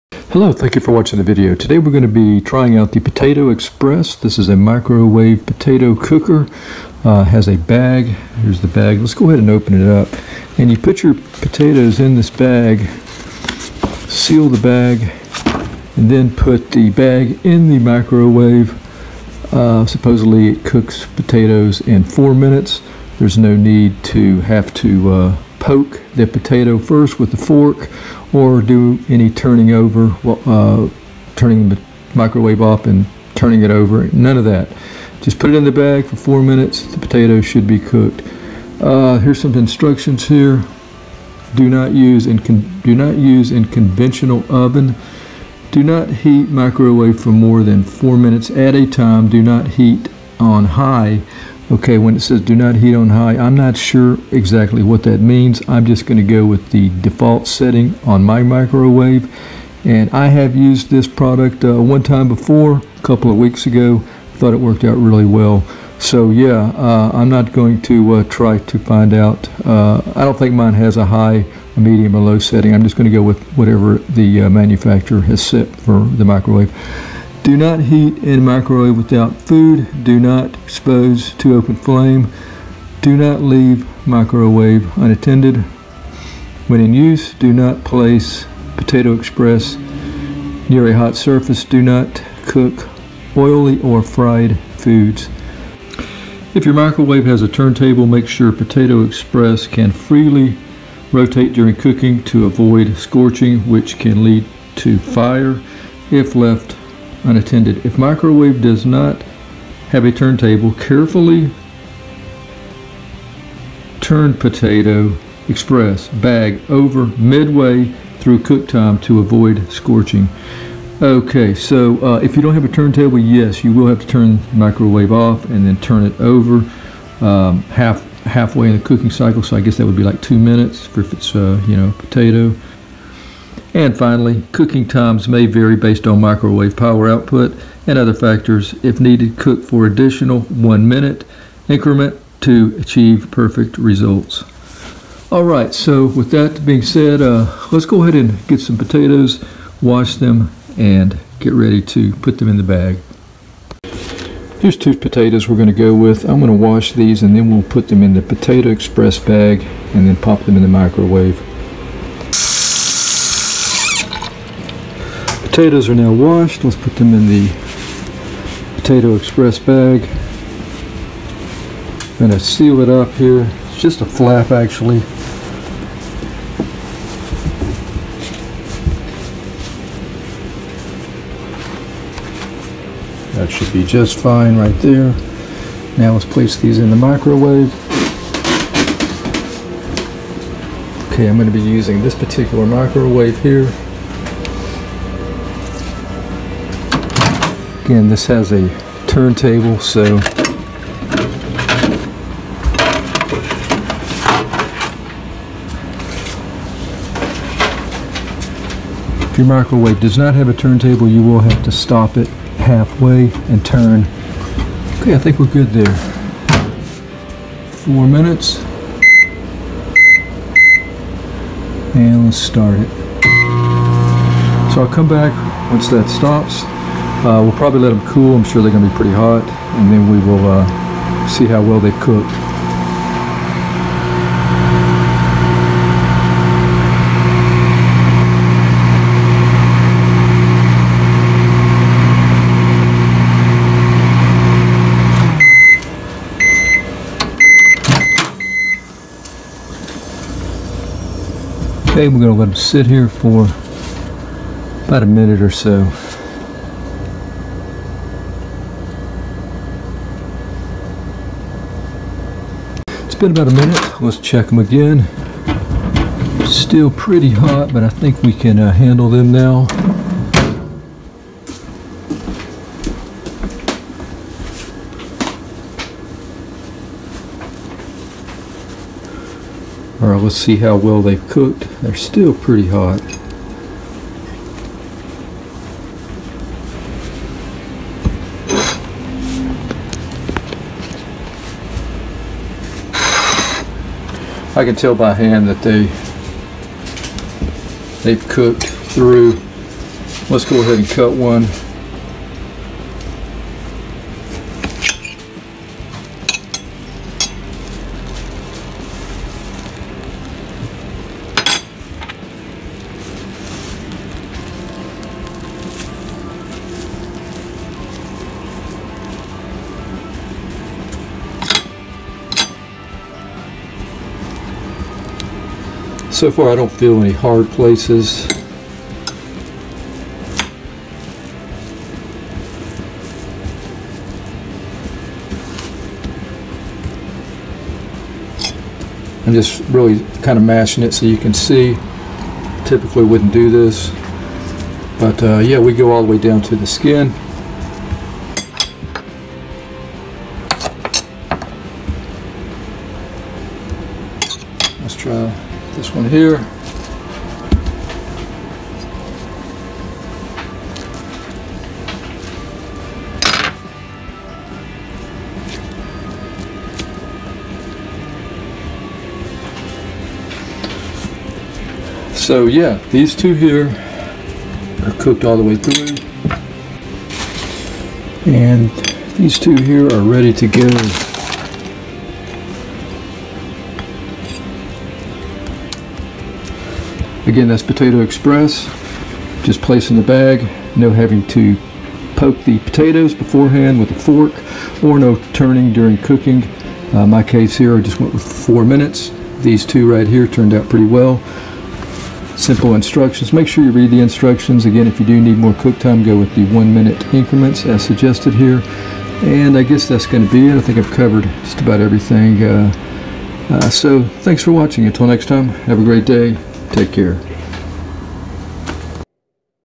Microwave Baked Potatoes in 4 minutes w/ POTATO EXPRESS?
I have tried this product twice with our turntable microwave & the results have been the same...